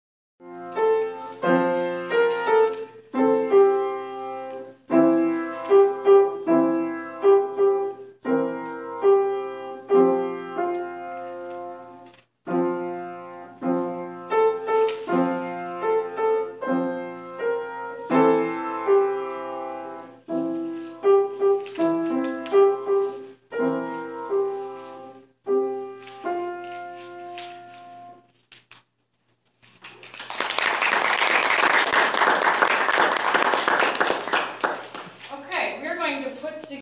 Shlok at World Music Instruments Day at PMA - Part 8
Shlok and his friends participating at PMA's world music day performance.